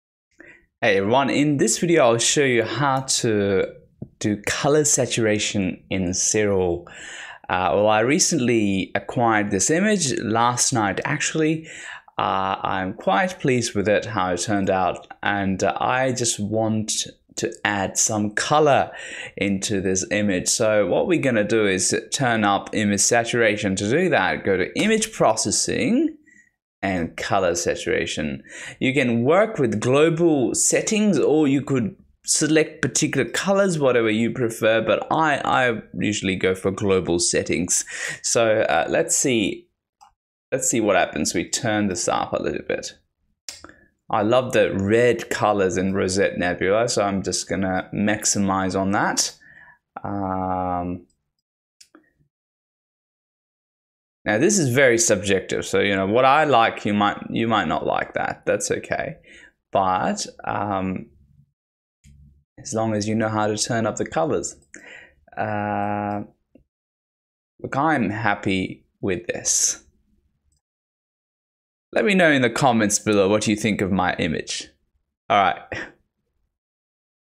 How to Bring Out Colors in SiriL -Astrophotography Tutorial
In this quick Astrophotography tutorial, you will see how to bring out colors in SiriL.